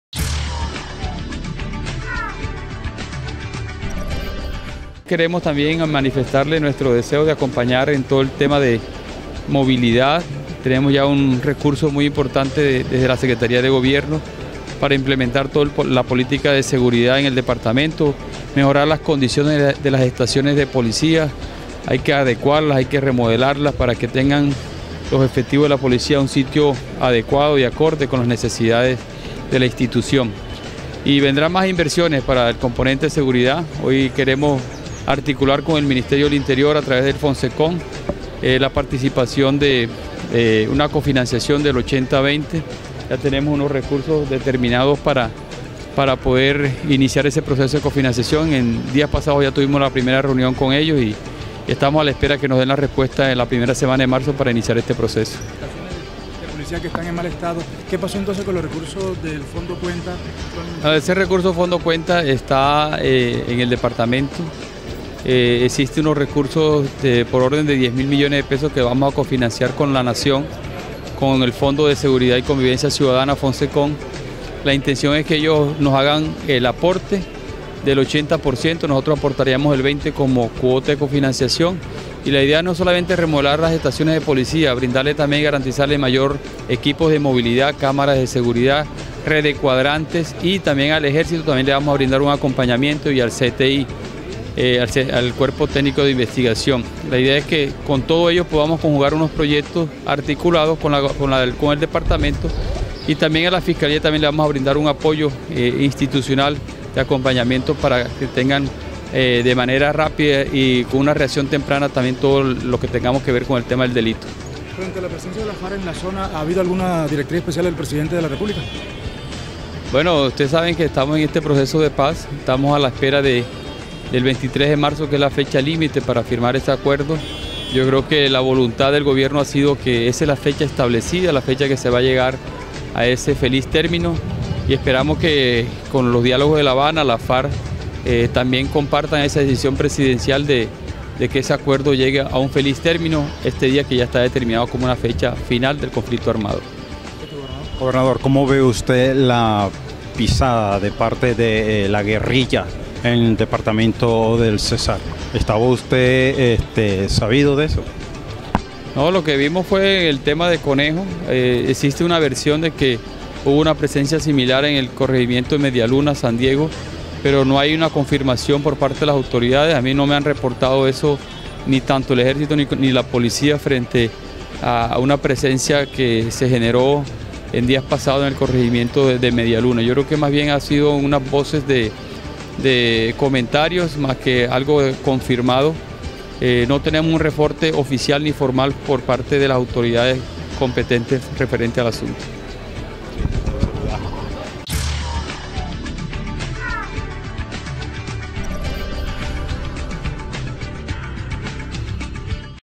Todo listo para la compra del lote para la sede de la Policía Metropolitana en Valledupar - Ovalle
Gobernador Franco Ovalle anunció compra de lote para construcción de sede de la Policía Metropolitana en Valledupar